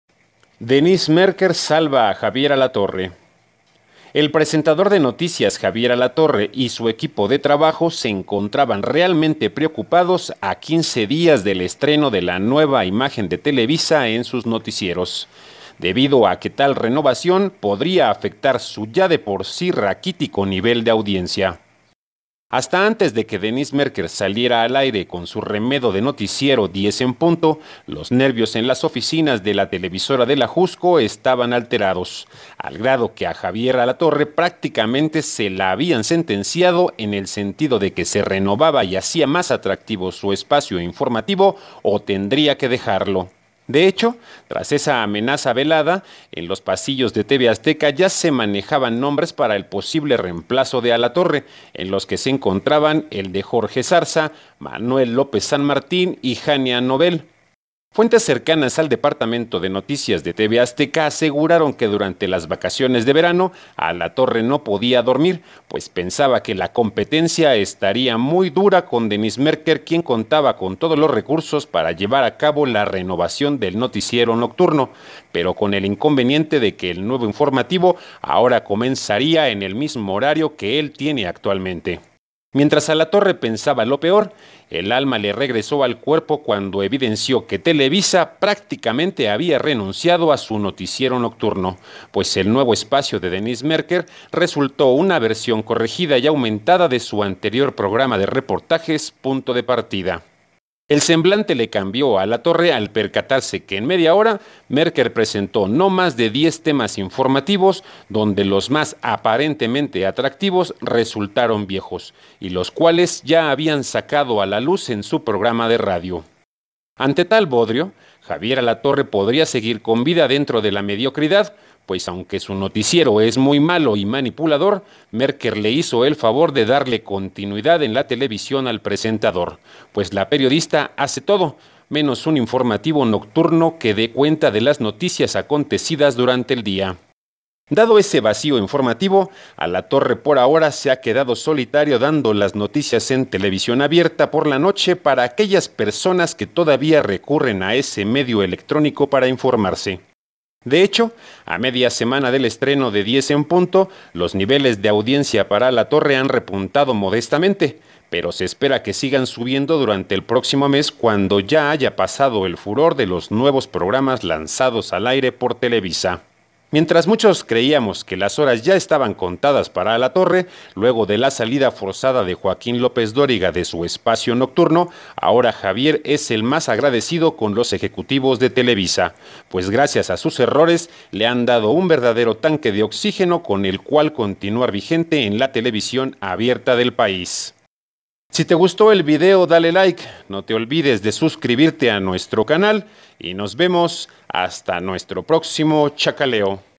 Denise Maerker Salva a Javier Alatorre
El presentador de noticias Javier Alatorre y su equipo de trabajo se encontraban realmente preocupados a quince días del estreno de la nueva imagen de Televisa en sus noticieros, debido a que tal renovación podría afectar su ya de por sí raquítico nivel de audiencia.